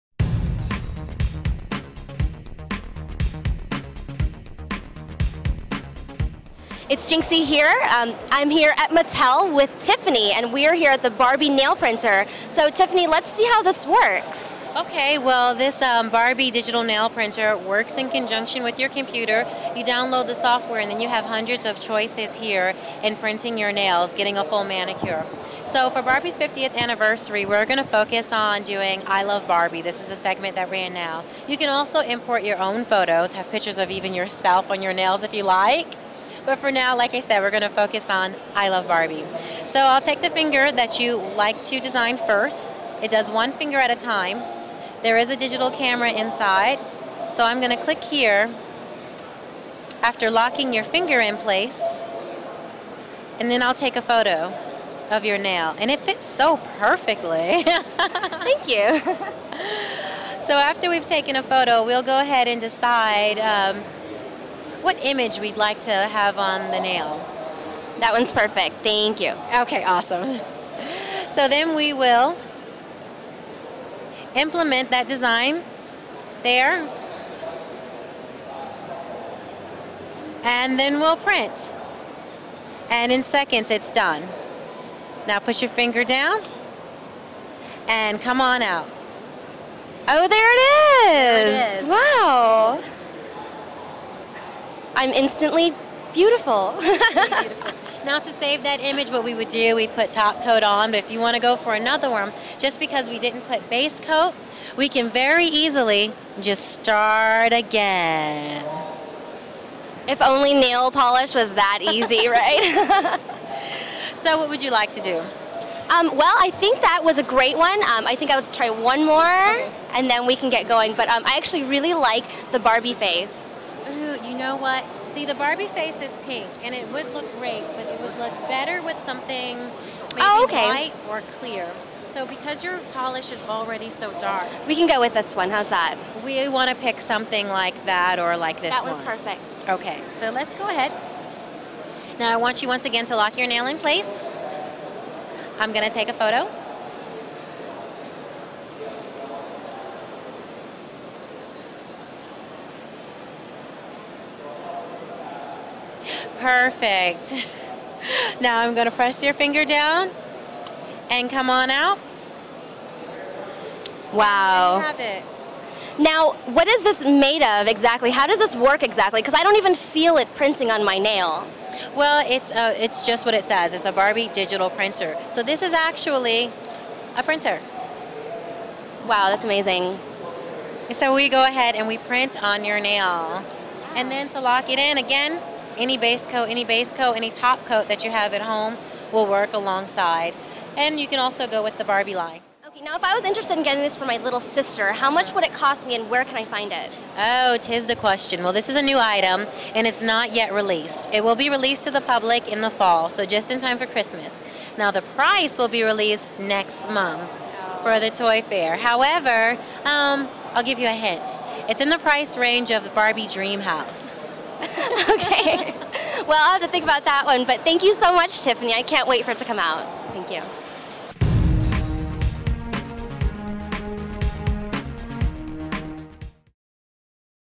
GEN CES '09 - Barbie Fingernails
Jinxie checks out the Barbie Fingernail Painter